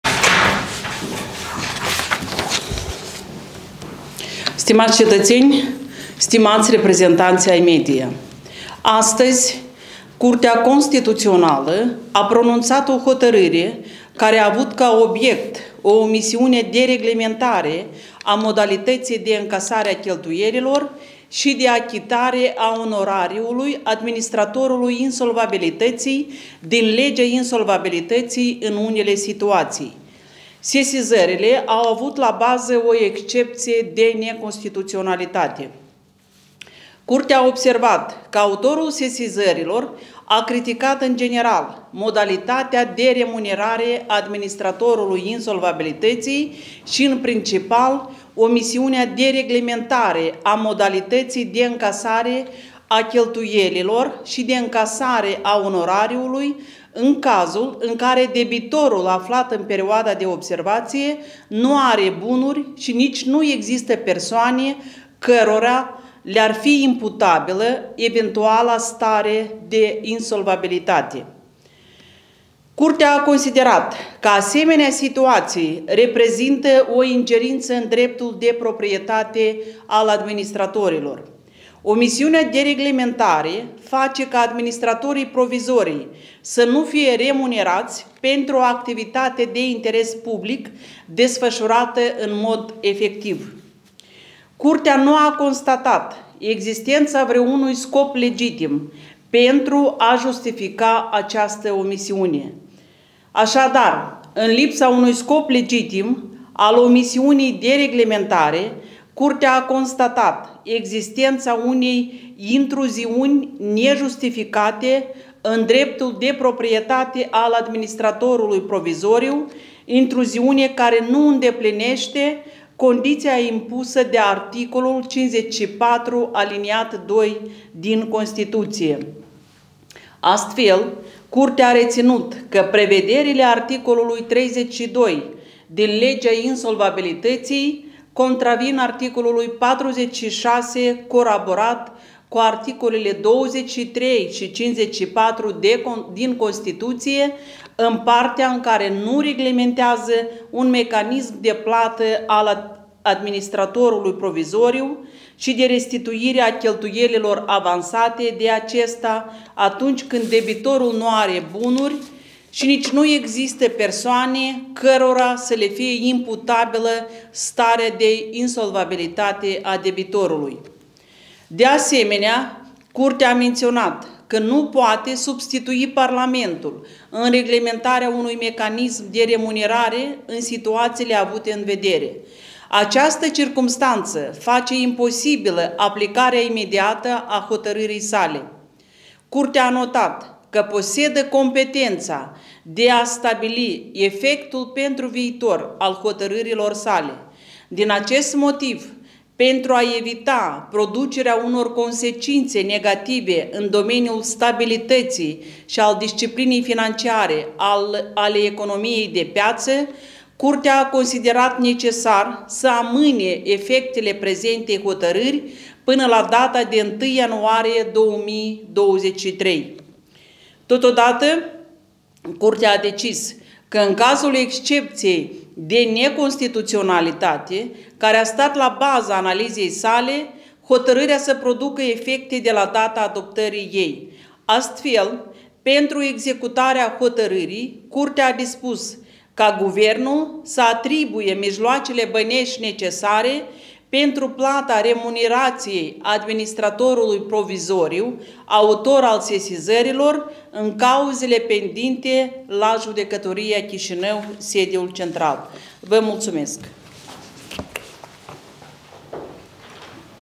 Briefing de presă susținut de Președintele Curții Constituționale, Domnica Manole - 5 aprilie 2022
Briefing de presă susținut de Președintele Curții Constituționale a Republicii Moldova, Domnica Manole - 5 aprilie 2022